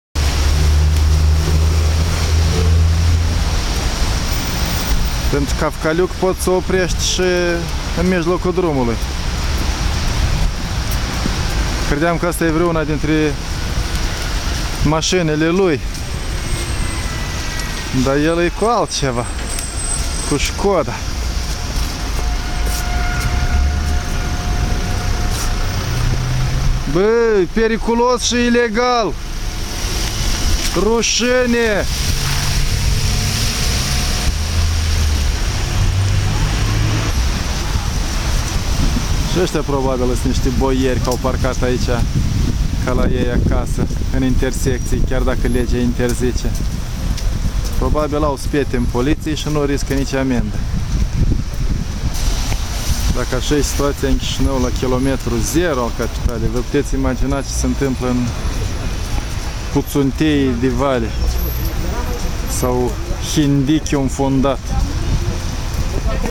Maşina lui Cavcaliuc staţiona ilegal în intersecţie - Curaj.TV
Dacă eşti grangulr din poliţie sau ai rude la primărie, în guvern poţi ignora şi legile gravitaţiei. Aşa cred prea mulţi în Moldova, de asta nu trăim ca în Europa ci, mai degrabă, ca în urss